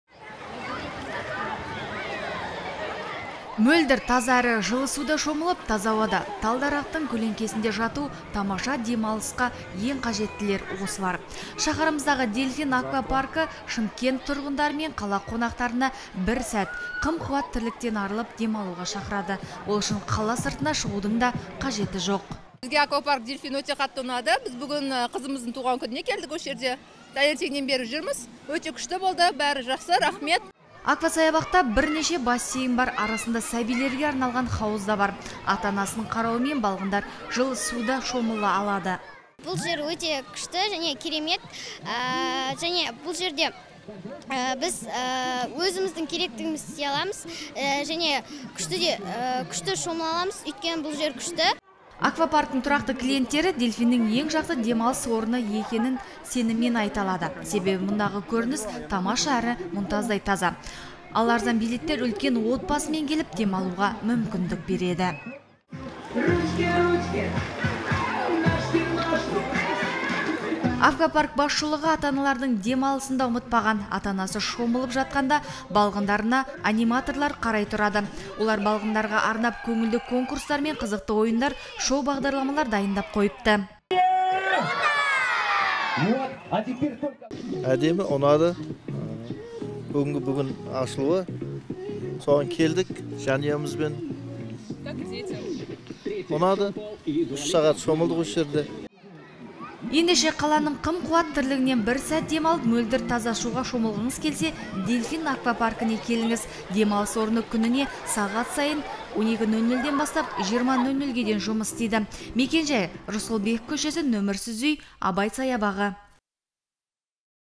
"Дельфин" аквабағы шақырады
Жаз келіп, аптап ыстық әбден күшіне мінді. Оның үстіне жазғы шілде де келіп қалды. Осындай сәтте салқын суға бір сүңгіп алуды кім қаламайды дерсіз? Ал Шымкентте осындай бір орын бар. Онда уақытты көңілді өткізіп, жақсылап тынығуға болады. Ол үшін алысқа бармай-ақ, "Дельфин" аквапаркіне келсеңіз болғаны. Жерлестеріміздің бұл демалыс орнына сеніммен келіп жүргеніне 15 жыл да болып қалыпты.